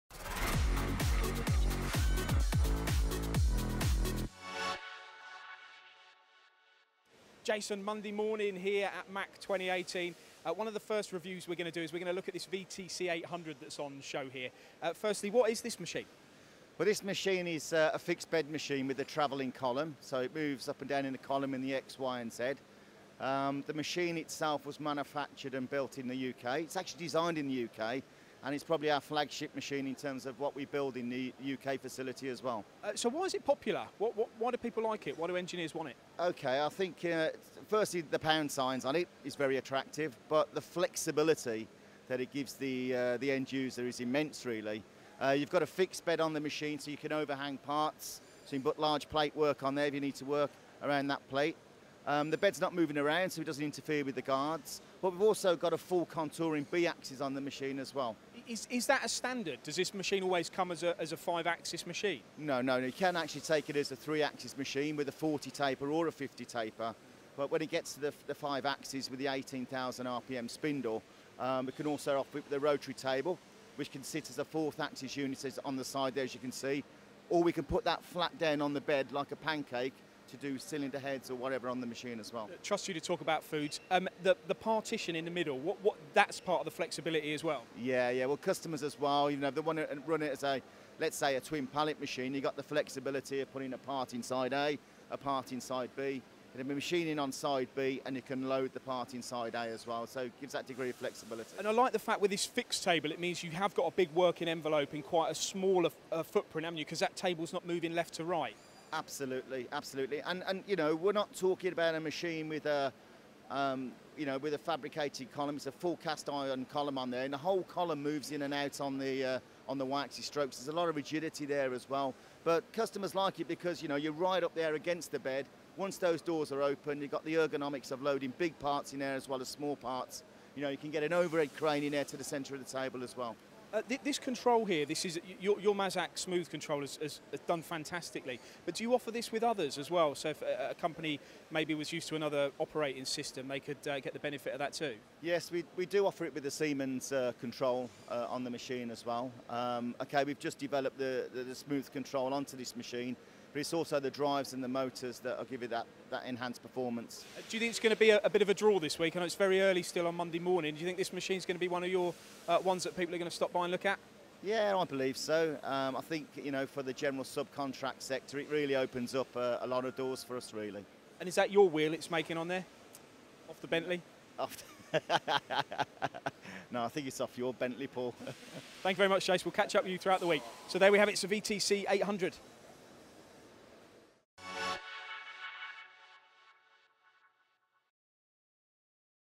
Jason Butler from Mazak talks to Paul about the VTC-800 on show this week at MACH 2018
MTD at MACH 2018: - we go live every hour at the UK's must-visit event for engineers and manufacturers...

Visit our MACH micro-site for more videos from this year's show.